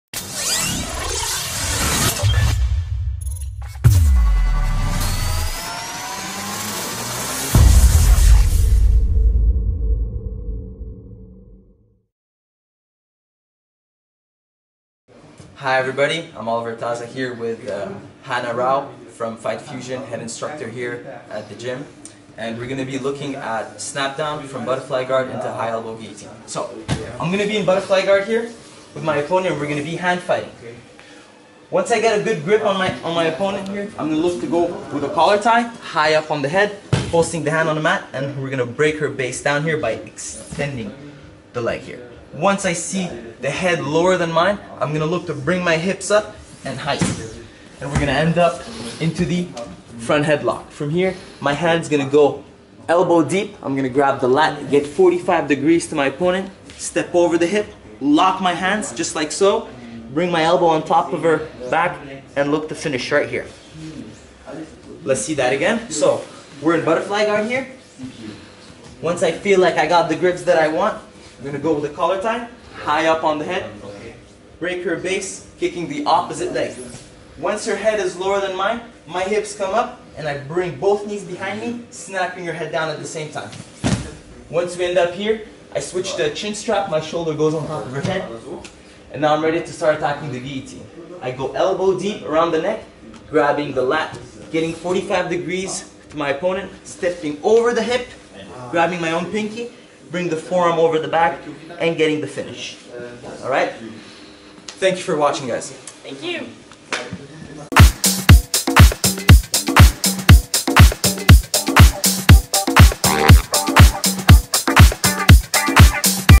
Oliver Taza - snap down from butterfly into high elbow guillotine
Guest instructor Oliver Taza shows a nice technique for our youtube followers. Come and visit us. BJJ & Grappling Germany - Fight Fusion Academy